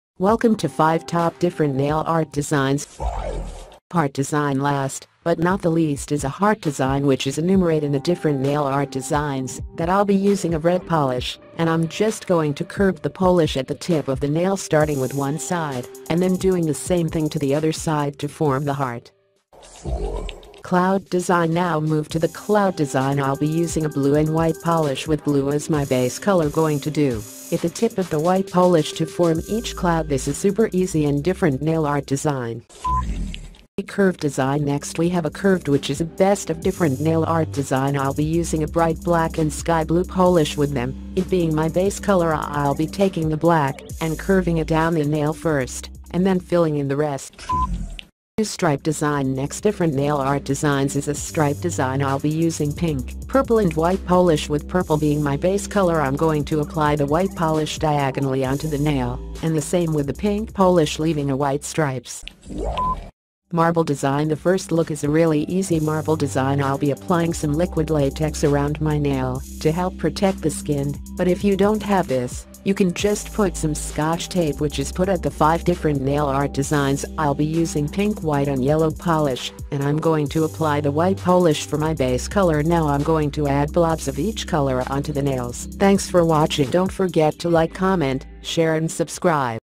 5 Top Different Nail Art Designs Best Nail Art Designs in the world
They have utilized no less than 5 distinct hues in this specific plan so it's. These nail craftsmanship plans can give a shocking look to your nails and can compel your. 5 Different Nail Art Designs I'll be utilizing pink white and yellow clean and will apply the white clean for my base shading now will include Blobs of each shading onto the nails. stripe plan I'll be utilizing pink, purple and white clean with purple being my base shading

we have a bended which is a best of various nail craftsmanship outline I'll be utilizing a brilliant dark and sky blue clean.

Presently move to the cloud outline I'll be utilizing a blue and white clean with blue as my base shading going to do if the tip of the white clean to frame each Cloud this is super simple and distinctive nail craftsmanship plan.

Last however not the minimum is a heart outline which is specify in the diverse nail craftsmanship plans.

Check for my Previous video:-

Check  for my Next video:-
5 Foods Suddenly Everywhere in the News - Most Rare Foods Sudden Explain News in the world!!